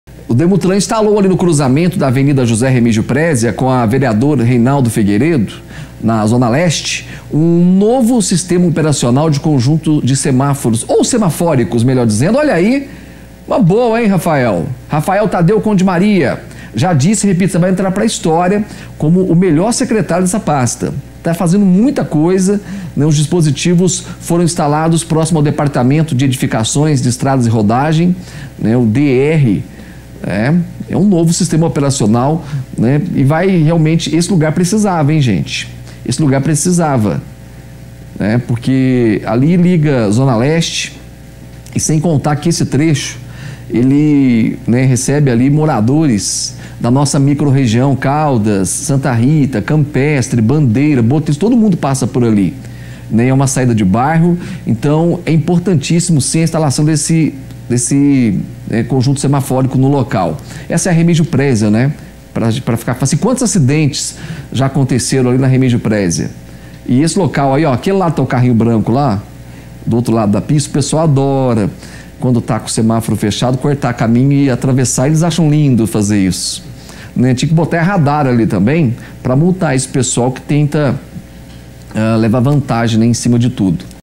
ÁREA EXTERNA DA UPA RECEBE ILUMINAÇÃO EM LED
De acordo com a prefeitura, as lâmpadas de LED são mais econômicas e possuem vida útil maior.